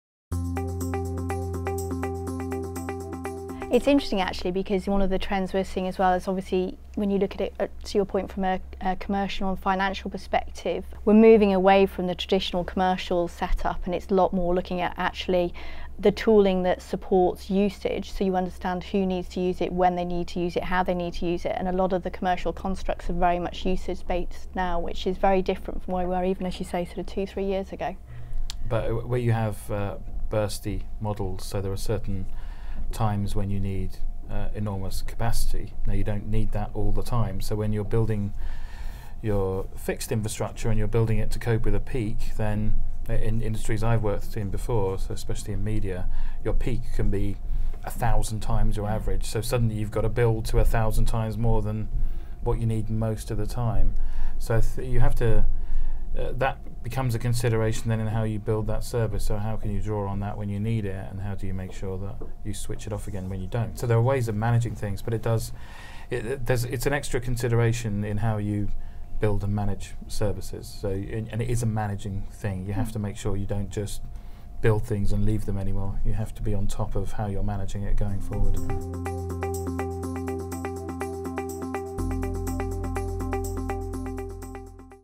What is cloud management and why is important for your 2020 plan?
Emma Roscow, Accenture UKI's Intelligent Cloud Infrastructure Lead, reveals why in this short extract from the Realising Cloud Value roundtable. Filmed at The Studio 2019, partnered with Accenture.